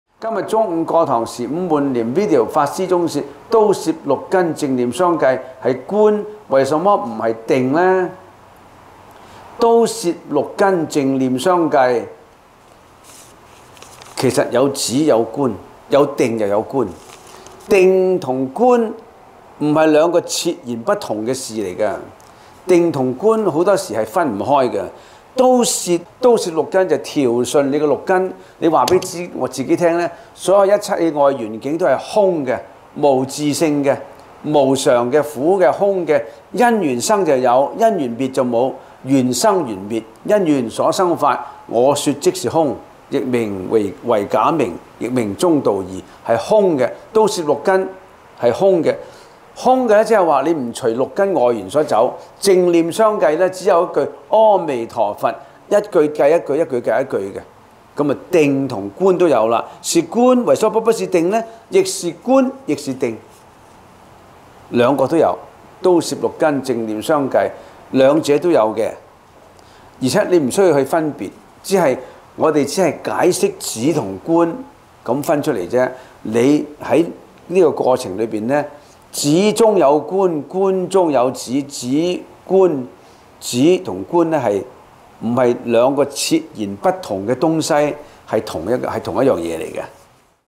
「都攝六根，淨念相繼」是觀還是定呢？觀成法師開示 - 佛法問與答 (部分有字幕)
「都攝六根，淨念相繼」是觀還是定呢？

佛法問與答系列
觀成法師 開示(部分有字幕)
觀看本系列所有影片請按：

本視頻原載於國際佛教觀音寺的 YouTube 頻道，原為法師「佛法問與答」第8集(8B)。佛門網 蒙國際佛教觀音寺允許轉載，藉以弘揚佛法，特此鳴謝感恩！想看觀成法師視頻、文章和專題報道，請瀏覽